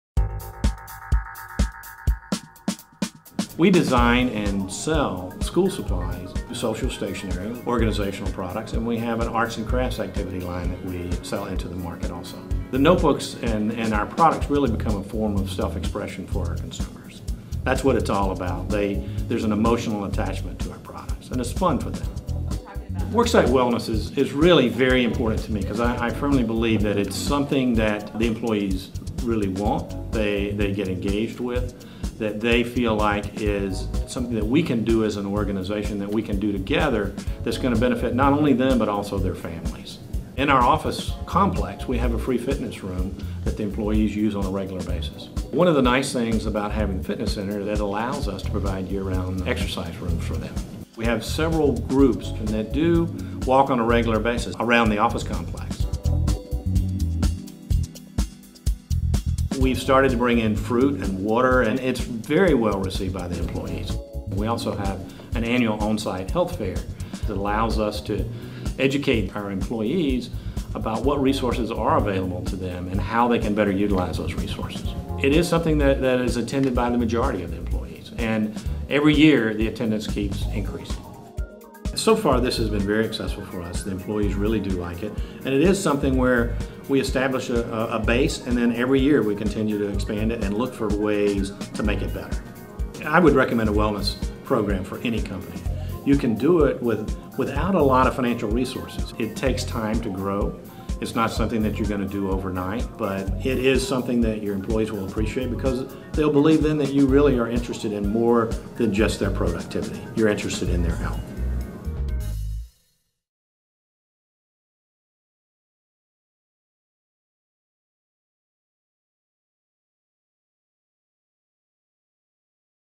Carolina Pad - A Worksite Wellness Success Story.mov
Located in Charlotte, Carolina Pad develops school and office supplies from concept to prototype.